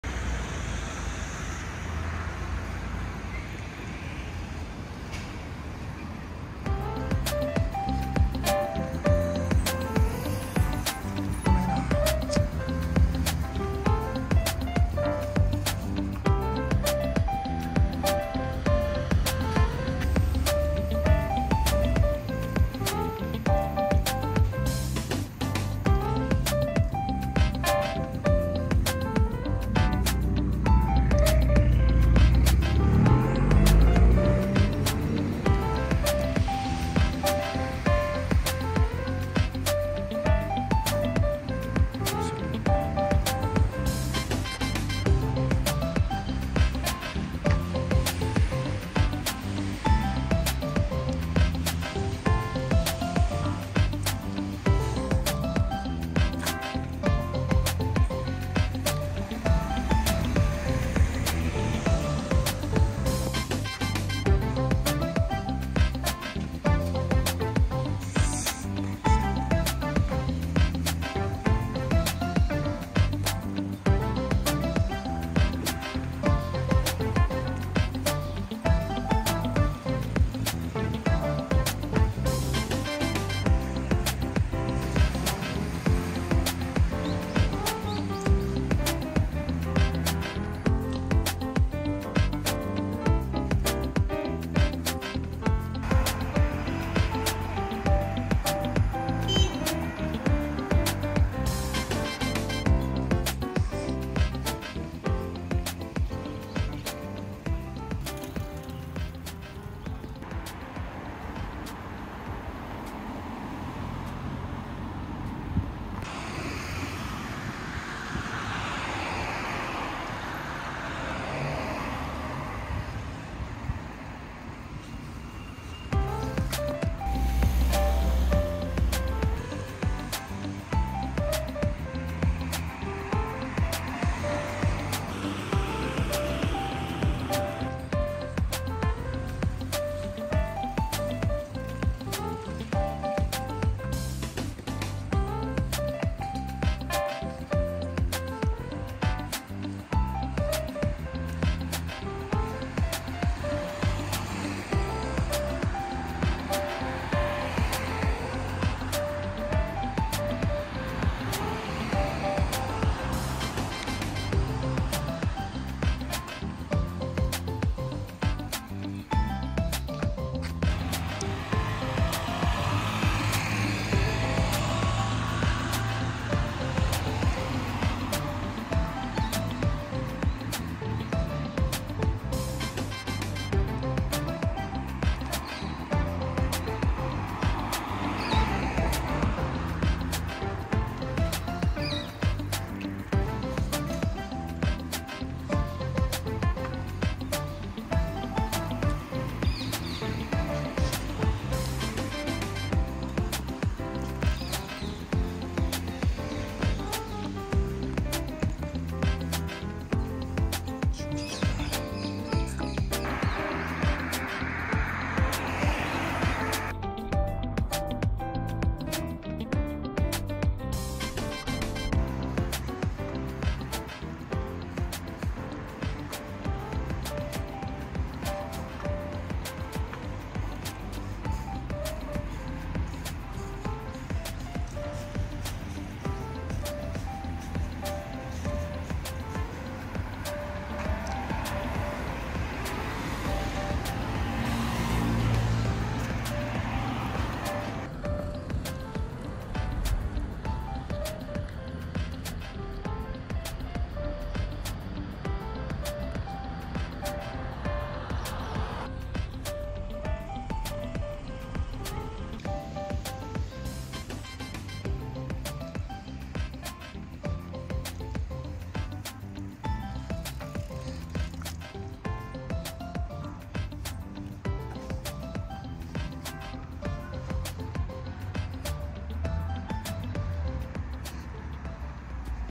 【Tokyo short trip】練馬春日町の桜並木 A row of cherry blossom trees in Nerima Kasuga-cho
練馬春日町の桜並木を歩きました。光が丘公園も少し。I walked along the cherry blossom trees in Nerima Kasuga-cho. Hikarigaoka Park is also a little.